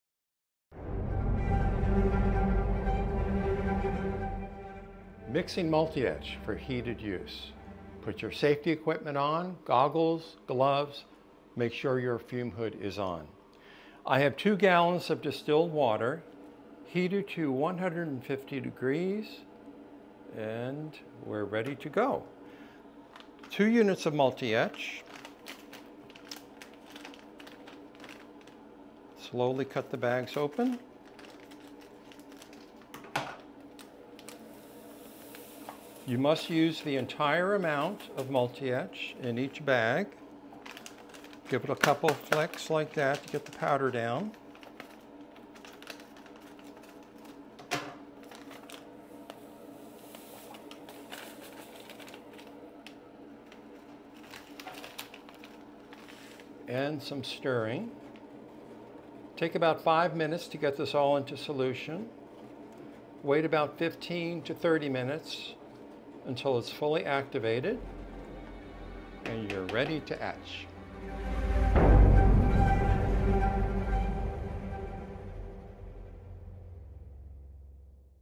Activating Multi-Etch For Heated Use
Activating your Multi-Etch is a one-time step that is required before you start etching. Chris Boothe walks you through the steps to prepare your etching bath, mix in Multi-Etch, and fully activate the solution.